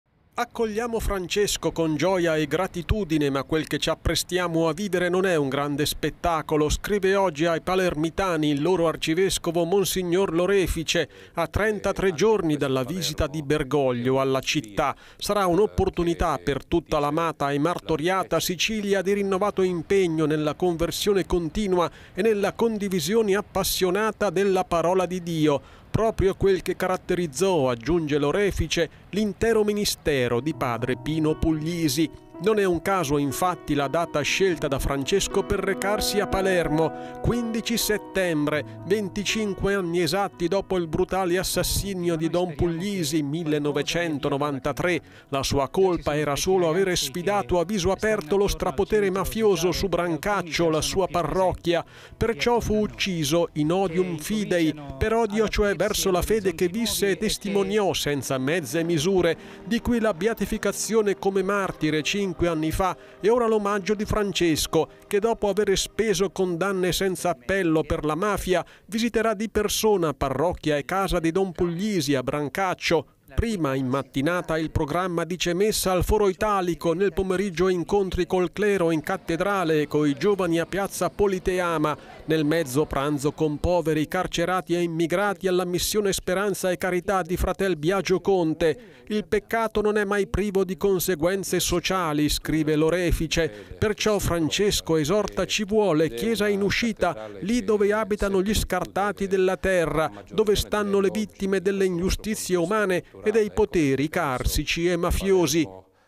Palermo si prepara ad accogliere Papa Francesco. Lettera pastorale di Mons. Lorefice
Palermo si prepara ad accogliere il Papa. Accadrà il 15 settembre in occasione del 25esimo anniversario dell'uccisione di don Puglisi. A presentare la prossima visita, in una lettera, l'arcivescovo di Palermo, monsignor Lorefice. Nello stesso giorno il papa visiterà anche piazza Armerina. Servizio di Paolo Fucili